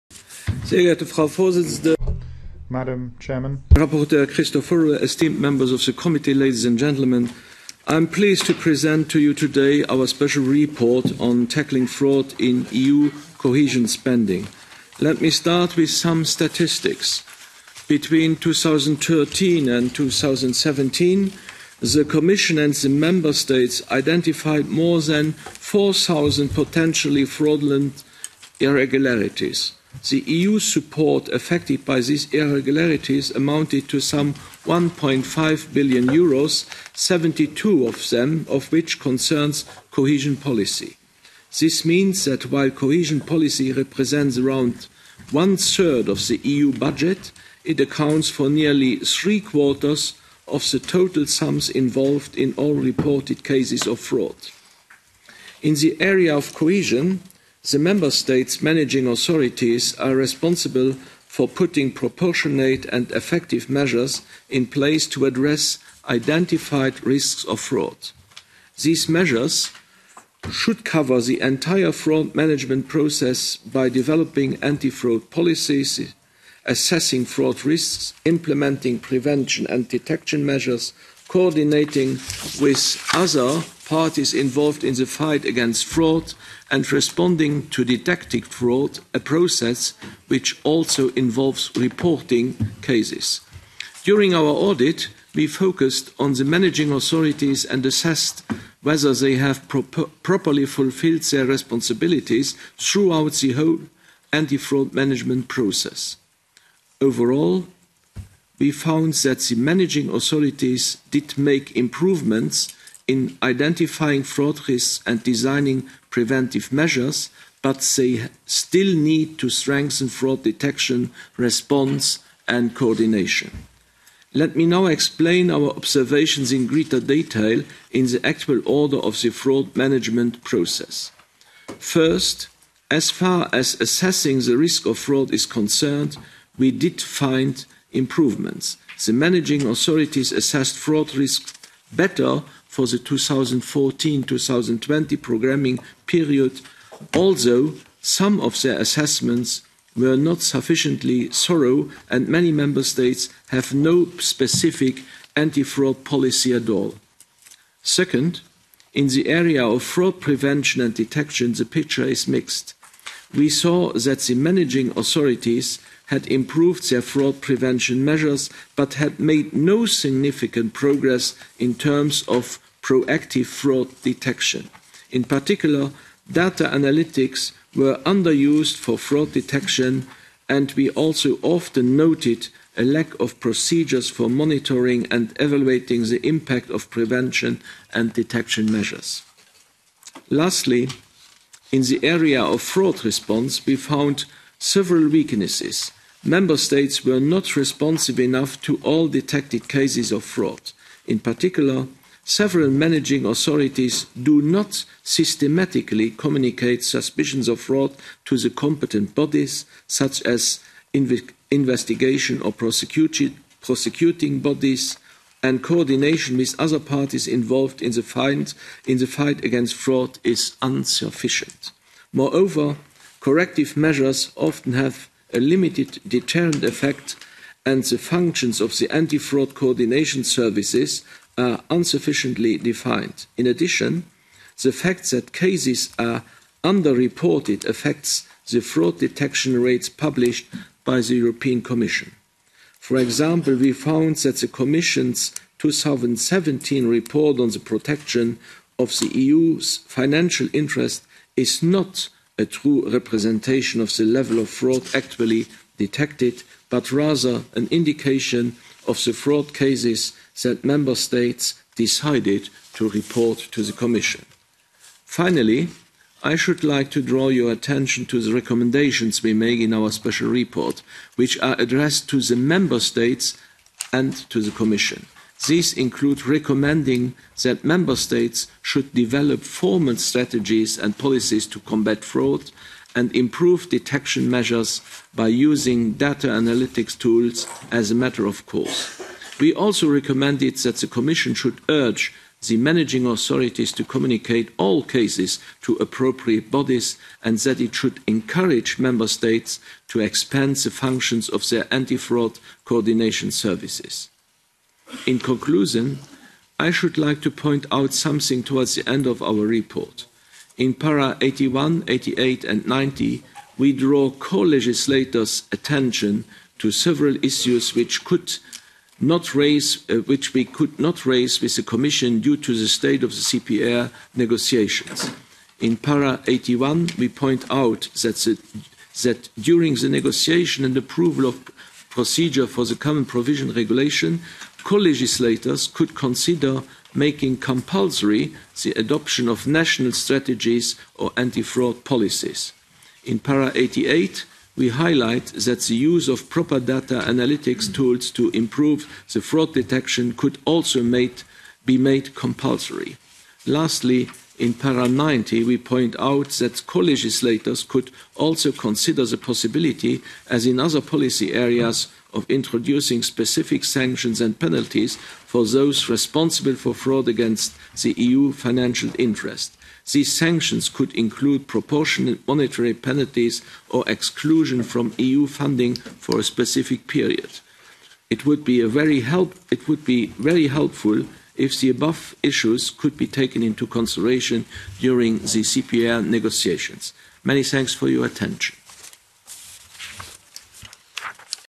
ECA presents a report on 'Tackling fraud in EUcohesion spending' to the European Parliament
🔴 This week, ECA Member Henri Grethen presented our special report on 'Tackling fraud in EUcohesion spending' to the EP's Budgetary Control Committee.